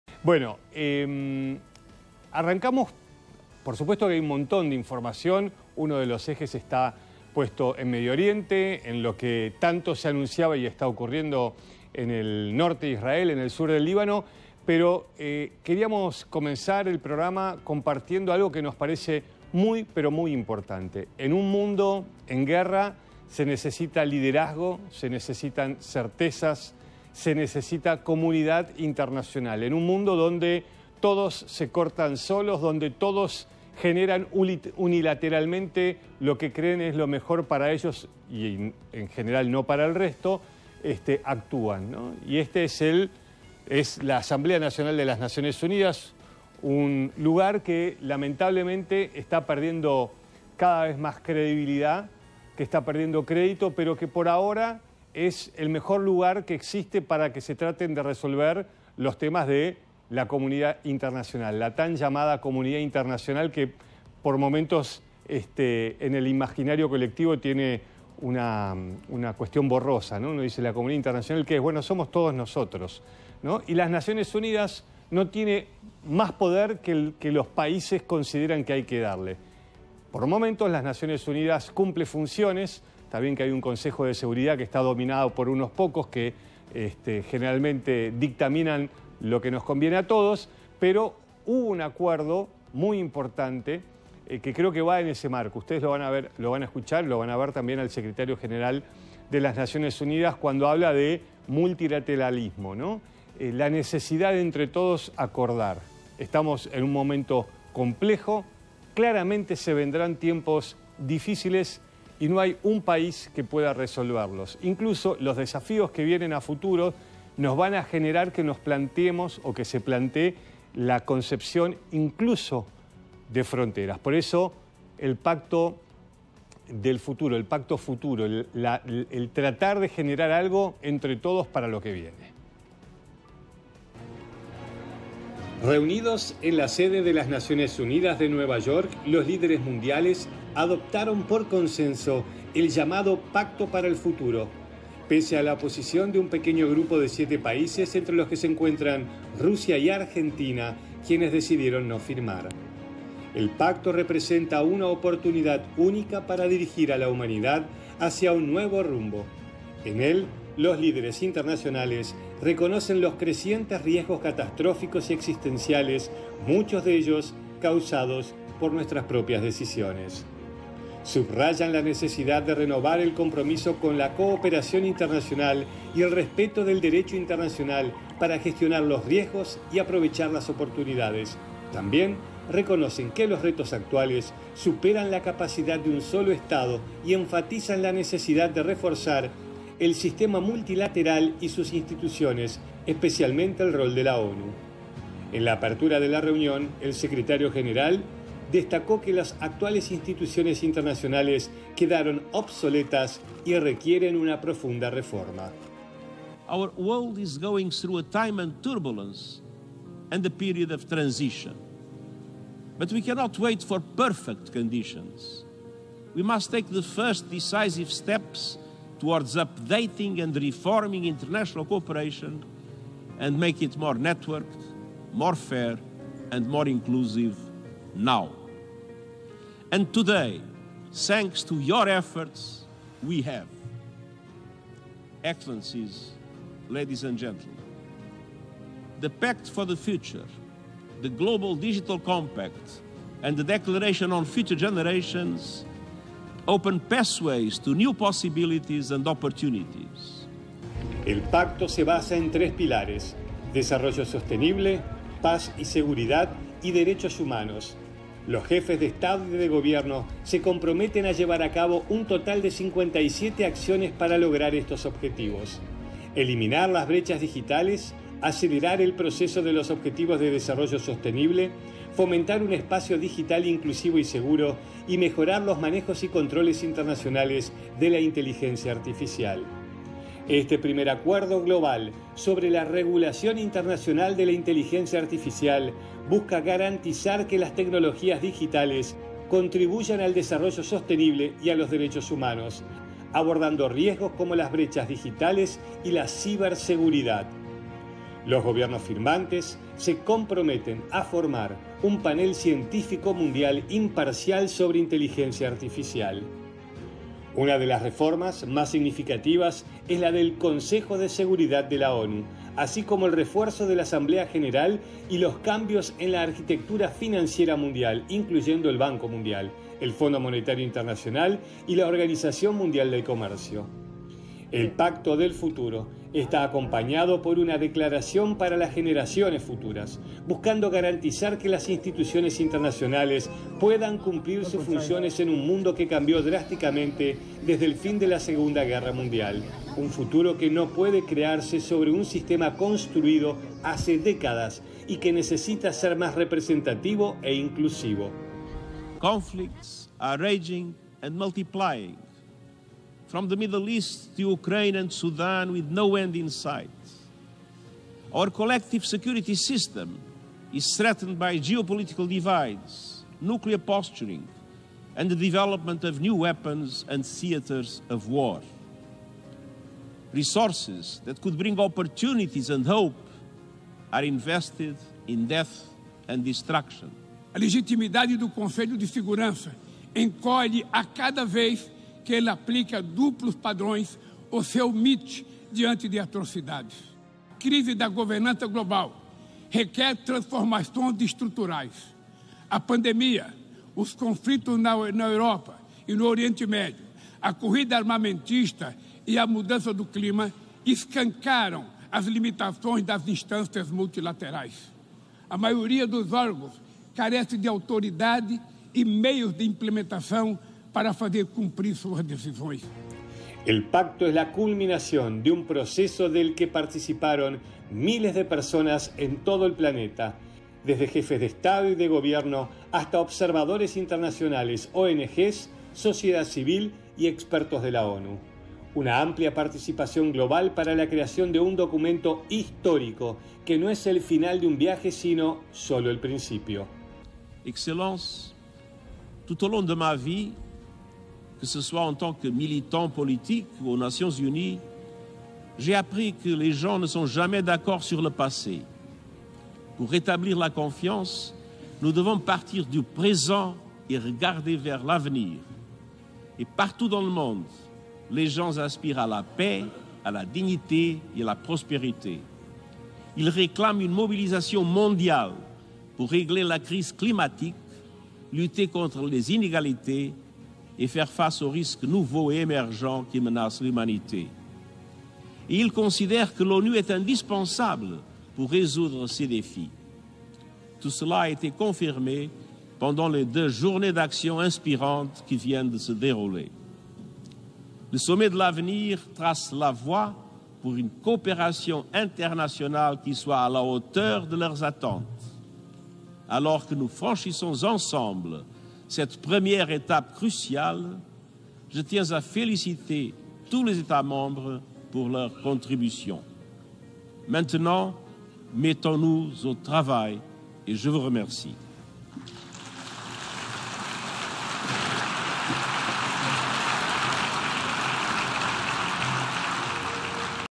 Pacto del Futuro: ¿de qué se trata el acuerdo global de las Naciones Unidas?
👉 Se trata de un documento de la ONU con 56 puntos que establece una visión y plan de acción para abordar desafíos globales, centrándose en 5 áreas principales: desarrollo sostenible; paz y seguridad internacionales; ciencia y tecnología; juventud y generaciones futuras. y transformación de la gobernanza mundial.